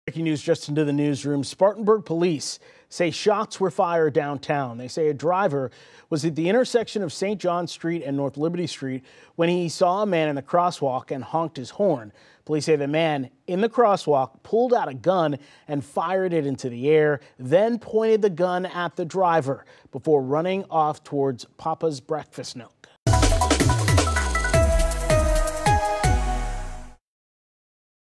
Man in crosswalk fires gun after driver honks at him, police say
Reports of shots fired near U of SC Upstate Monday evening have police searching for the shooter.